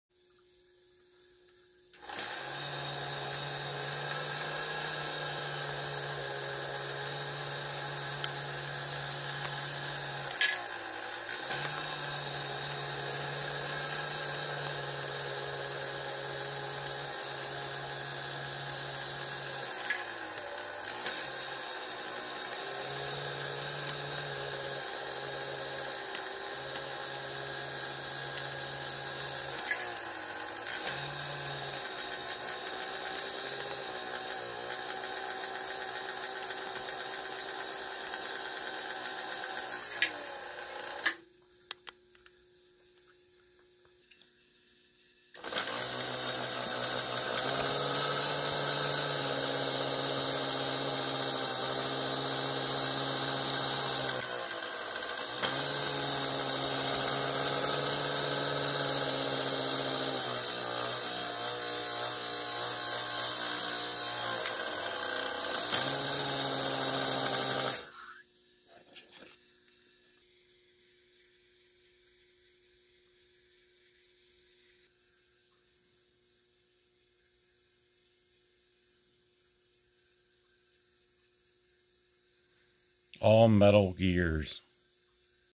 Sawmill American Flyer by Lionel.
Check for all metal gears.  This came in orange Lionel box.  Some in A.F. blue packaging  had plastic gear which wore out.  Check with Port Lines Hobby Supplies for brass replacement.  String at bit tight.